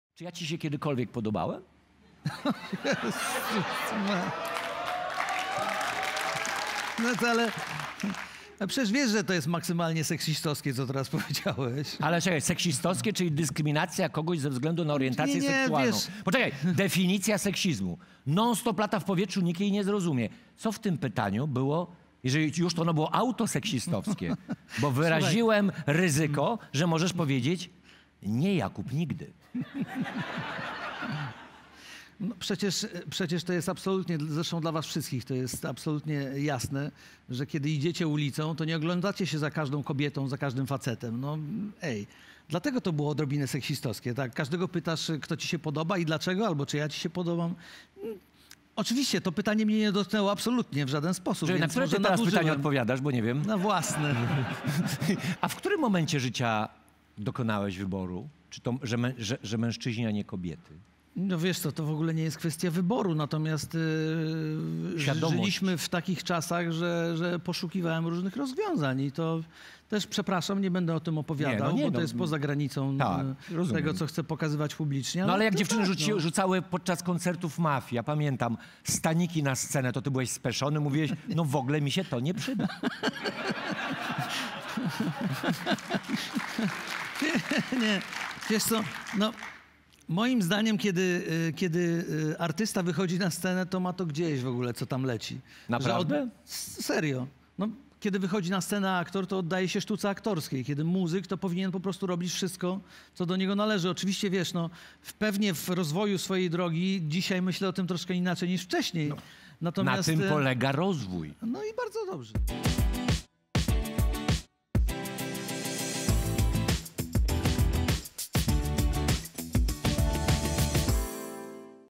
Wojewódzki do Piasecznego: "Czy ja Ci się kiedykolwiek podobałem?"
Już we wtorek na legendarnej i skórzanej kanapie zasiądzie piosenkarz Andrzej Piaseczny (u którego jak twierdzi pewien profesor nie tylko entuzjazm jest zaraźliwy) oraz raperka, piosenkarka, realizatorka dźwięku i DJ-ka Young Leosia Zapraszamy!